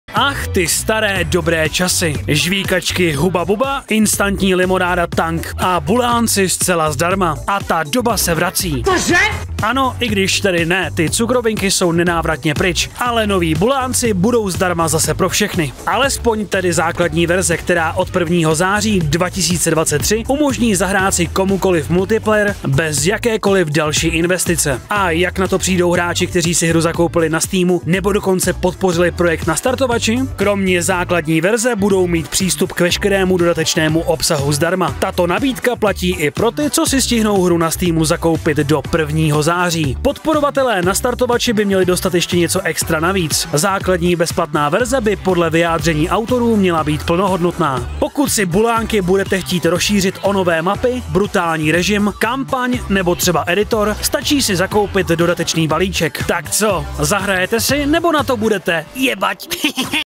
Bulánci budou zdarma! Co ale dostanou ti, kteří už zaplatili? 🤑
Nové Bulánky čeká důležitý měsíc. 1. srpna 2023 začíná online multiplayer a o měsíc později budou bitvy polštářů zdarma pro všechny hráče. Půjde o základní verzi, kterou si bude moci zahrát každý.

Děkujeme obchodu s elektronikou CZC.cz